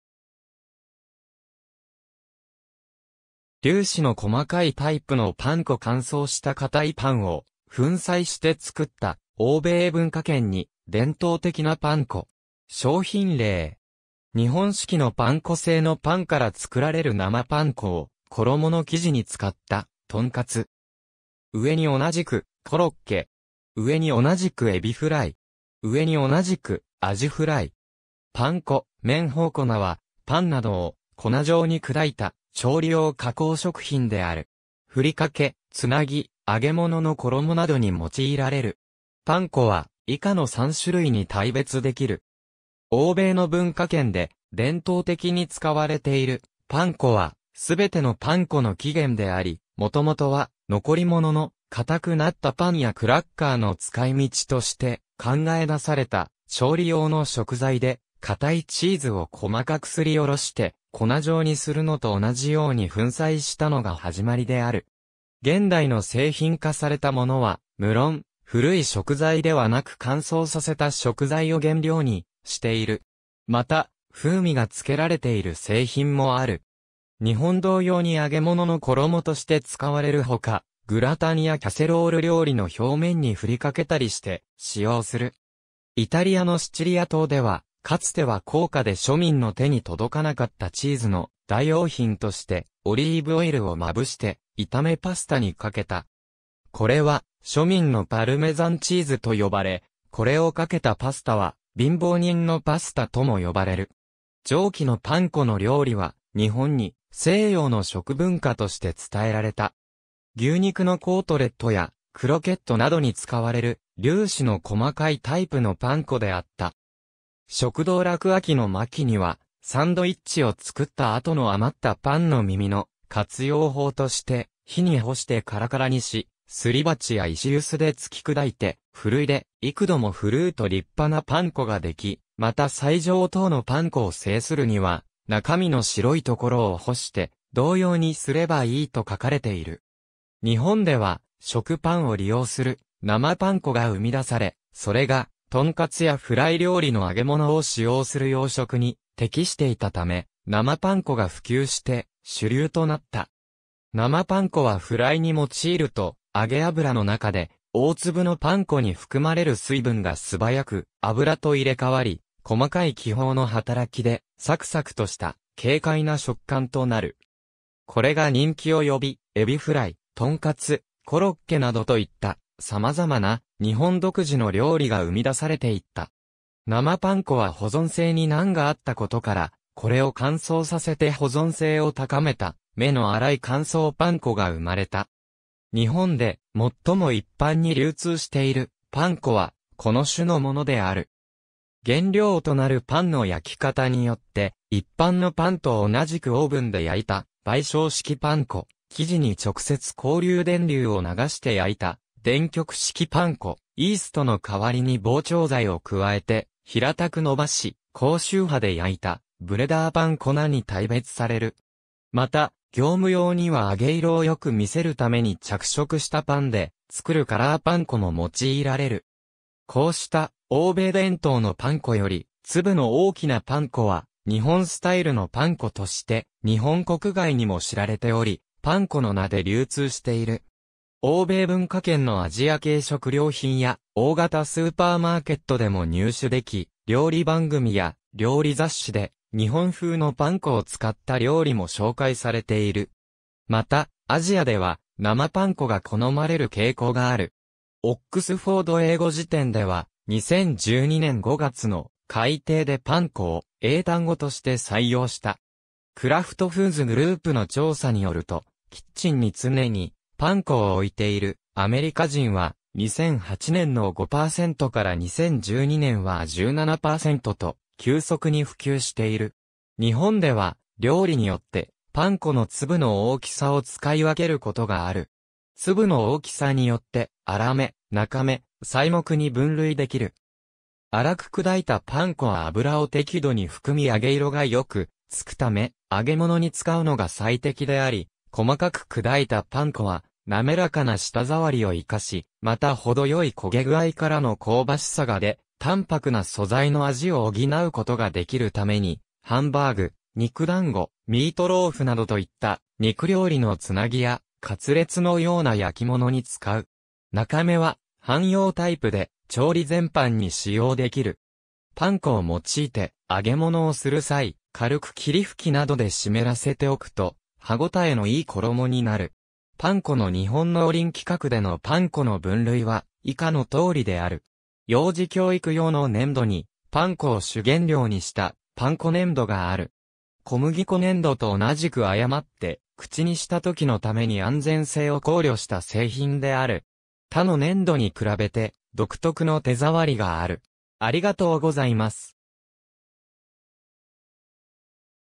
パン粉
粒子の細かいタイプのパン粉 乾燥した硬いパンを粉砕して作った、欧米文化圏に伝統的なパン粉。
商品例（スウェーデン製）。
日本式のパン粉 生のパンから作られる生パン粉を衣の生地に使った「豚カツ」。
上に同じく、コロッケ。
上に同じくエビフライ。
上に同じくアジフライ。
パン粉、麺麭粉（パンこ）は、パンなどを粉状に砕いた調理用加工食品である。
ふりかけ、つなぎ、揚げ物の衣などに用いられる。
パン粉は、以下の3種類に大別できる。
欧米の文化圏で伝統的に使われているパン粉（英: bread crumbs, breadcrumbs, 仏: chapelure）は、全てのパン粉の起源であり、元々は残り物の硬くなったパンやクラッカーの使い道として考え出された調理用の食材で、硬いチーズを細かくすりおろして粉状にするのと同じように粉砕したのが始まりである。
現代の製品化されたものは、むろん、古い食材ではなく乾燥させた食材を原料にしている。
また、風味が付けられている製品もある。
日本同様に揚げ物の衣として使われるほか、グラタンやキャセロール料理の表面にふりかけたりして使用する。
イタリアのシチリア島では、かつては高価で庶民の手に届かなかったチーズの代用品として、オリーブオイルをまぶして炒めパスタにかけた。
これは「庶民のパルメザンチーズ」と呼ばれ、これをかけたパスタは「貧乏人のパスタ」とも呼ばれる。
上記のパン粉の料理は、日本に西洋の食文化として伝えられた。
牛肉のコートレットやクロケットなどに使われる、粒子の細かいタイプのパン粉であった。
食道楽 秋の巻(1903～1904年)には、サンドイッチを作った後の余ったパンの耳の活用法として、日に干してカラカラにし、すり鉢や石臼でつき砕いて、ふるいで幾度もふるうと立派なパン粉ができ、また最上等のパン粉を製するには中身の白い処を干して同様にすればいいと書かれている。
日本では、食パンを利用する「生パン粉」が生み出され、それが豚カツやフライ料理の揚げ物を使用する洋食に適していたため、生パン粉が普及して主流となった。
生パン粉はフライに用いると、揚げ油の中で大粒のパン粉に含まれる水分が素早く油と入れ替わり、細かい気泡の働きでサクサクとした軽快な食感となる。
これが人気を呼び、エビフライ、豚カツ、コロッケなどといった様々な日本独自の料理が生み出されていった。
生パン粉は保存性に難があった事から、これを乾燥させて保存性を高めた、目の荒い乾燥パン粉が生まれた。
日本で最も一般に流通しているパン粉は、この種のものである。
原料となるパンの焼き方によって、一般のパンと同じくオーブンで焼いた焙焼式パン粉、生地に直接交流電流を流して焼いた電極式パン粉(電気パン)、イーストの代わりに膨張剤を加えて平たく伸ばし高周波で焼いたブレダーパン粉に大別される。
また、業務用には揚げ色をよく見せるために着色したパンで作るカラーパン粉も用いられる。
こうした、欧米伝統のパン粉より粒の大きなパン粉は、「日本スタイルのパン粉 (Japanese style breadcrumbs)」として日本国外にも知られており、「panko」 の名で流通している。
欧米文化圏のアジア系食料品や大型スーパーマーケットでも入手でき、料理番組や料理雑誌で日本風のパン粉を使った料理も紹介されている。
また、アジアでは生パン粉が好まれる傾向がある。
オックスフォード英語辞典では、2012年5月の改訂で Panko を英単語として採用した。
クラフト・フーズ・グループの調査によると、キッチンに常にパン粉を置いているアメリカ人は、2008年の5%から2012年は17%と急速に普及している。
日本では料理によってパン粉の粒の大きさを使い分けることがある。
粒の大きさによって荒目、中目、細目に分類できる。
荒く砕いたパン粉（荒目）は油を適度に含み揚げ色がよくつくため、揚げ物に使うのが最適であり、細かく砕いたパン粉（細目）は、滑らかな舌触りを生かし、また程よい焦げ具合からの香ばしさが出、淡泊な素材の味を補うことができるために、ハンバーグ、肉団子、ミートローフなどといった肉料理のつなぎや、カツレツのような焼き物に使う。
中目は、汎用タイプで、調理全般に使用できる。
パン粉を用いて揚げ物をする際、軽く霧吹きなどで湿らせておくと歯ごたえのいい衣になる。
パン粉の日本農林規格でのパン粉の分類は以下のとおりである。
幼児教育用の粘土にパン粉を主原料にしたパン粉粘...